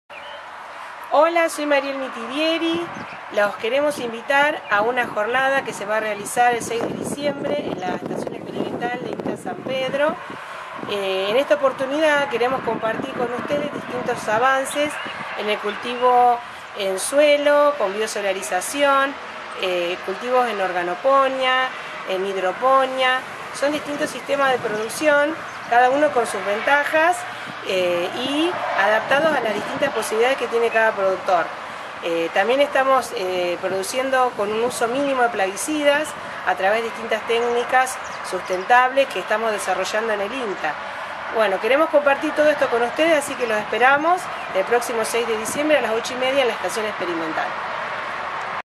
Producción sostenible de flores y hortalizas
Mariel Mitidieri invita a la jornada del 6 de diciembre para conocer diversas formas de producir cuidando el medio ambiente. Se compartirán experiencias de biosolarización, cultivos hidropónicos y organopónicos, y cosecha de agua, entre otros temas.